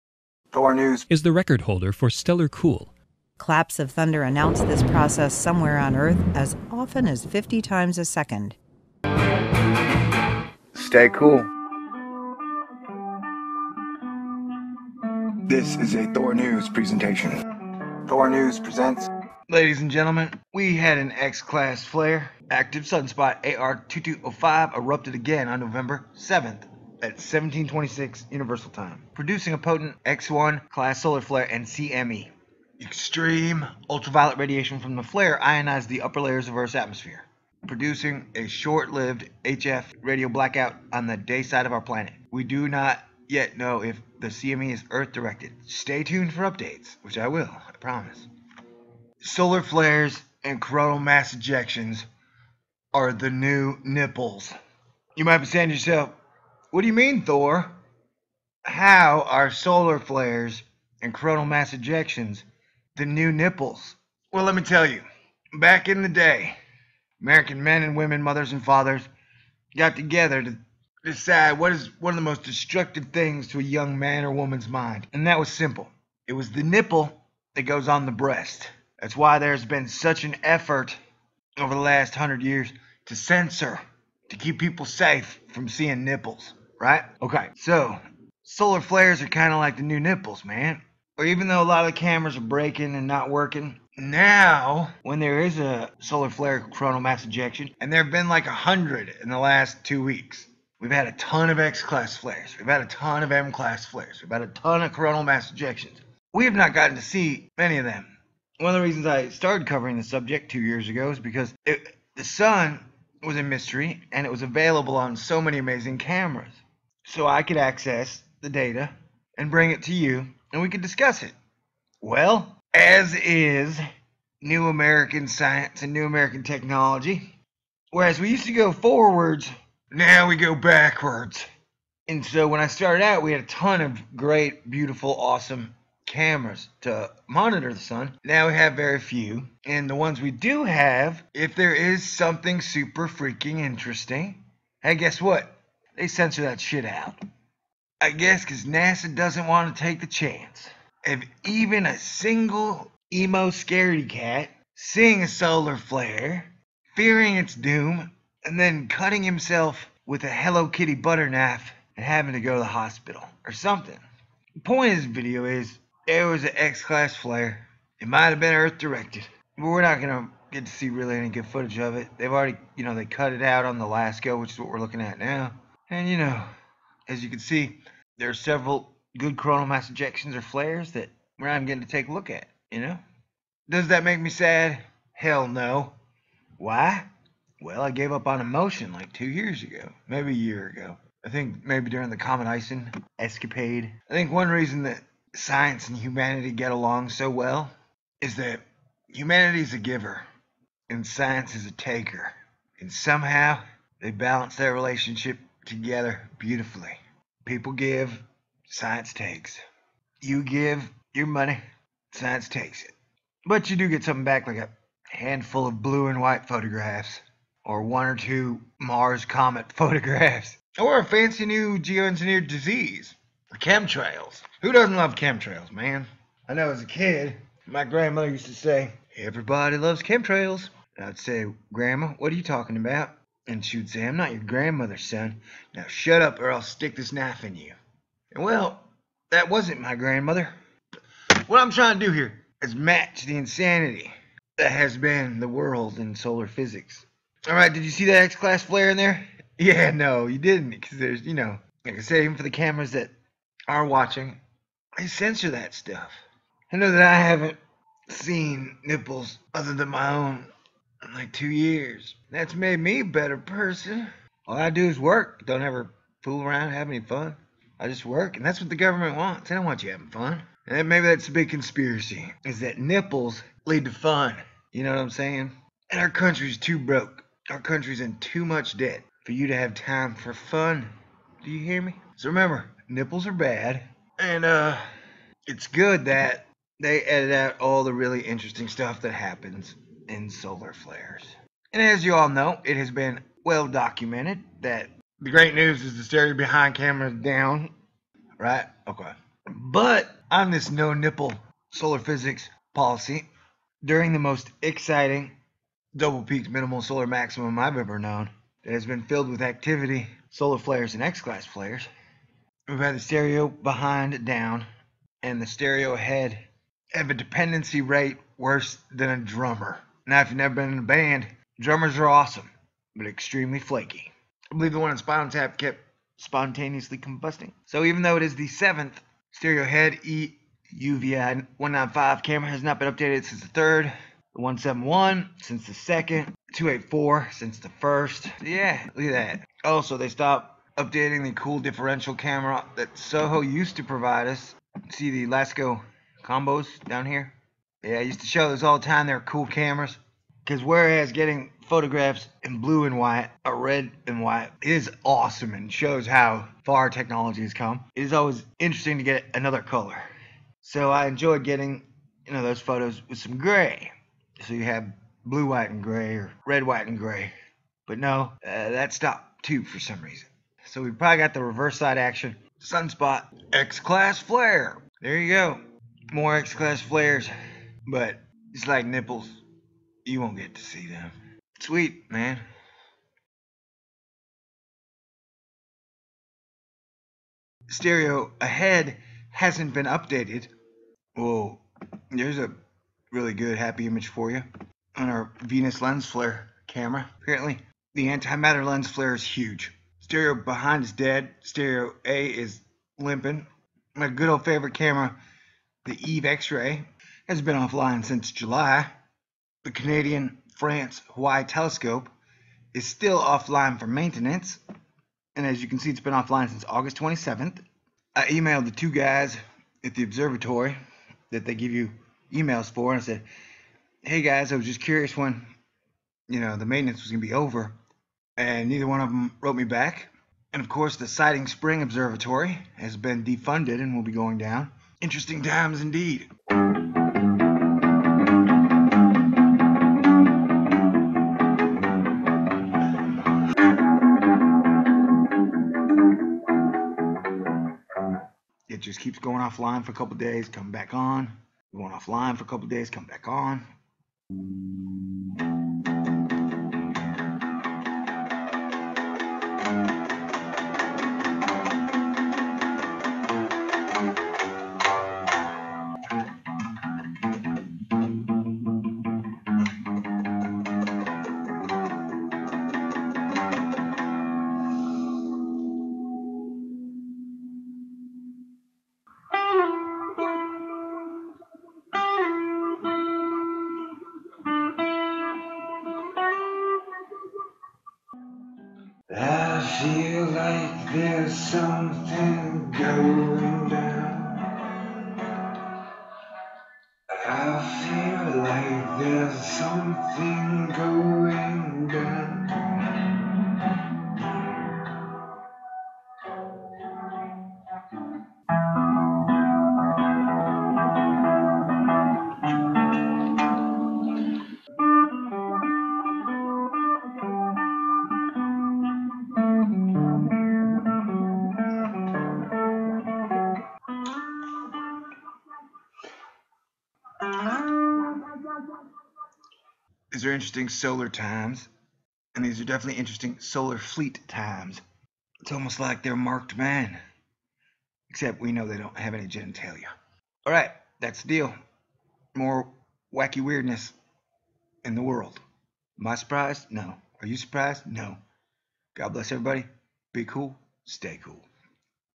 Possible Earth Directed X Class Solar Flare & CME's are the new Nipples
We've got an X 1 Class Solar Flare and Coronal Mass Ejection and I wax ecstatic about how Solar Flares, CMEs and Solar Activity in general is the new Nipple of Science and Astronomy.

T